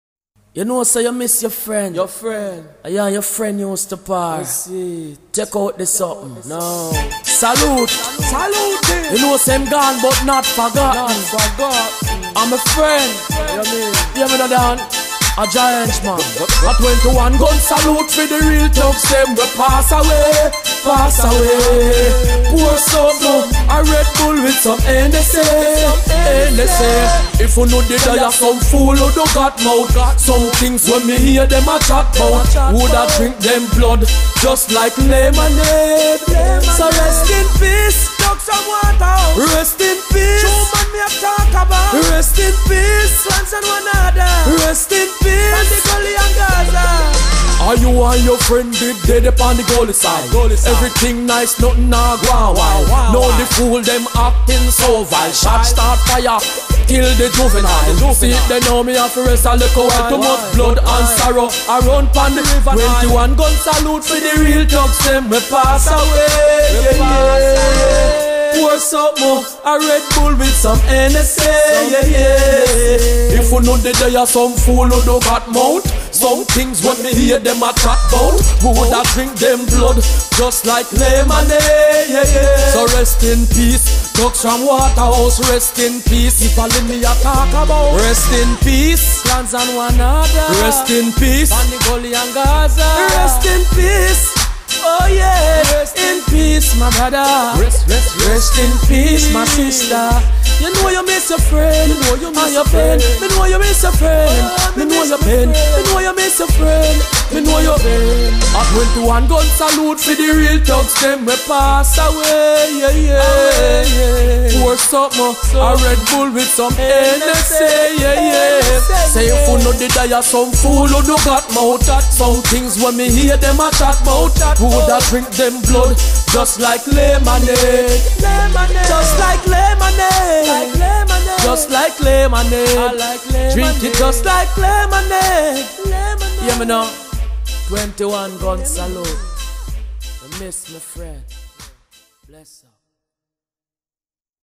21 gun salute_jah henchman_0001.wmv
Tribute to my good friend True man rest in piece, 21 gun salute, Jah bless your soul.